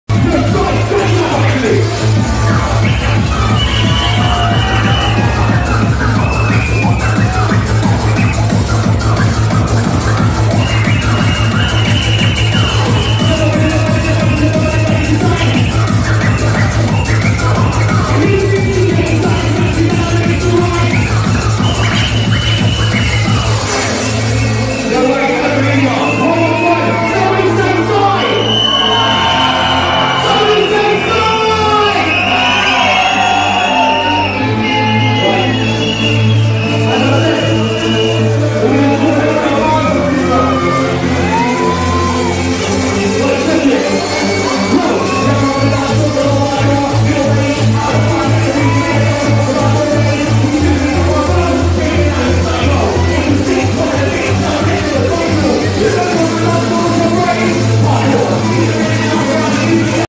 DJ SY, THE SCRATCH MASTER! WORKING THE DECKS ALONG WITH MC ENEMY
Search for THE RAVE ESCAPE on Facebook and Instagram to keep up to date with our events 🙏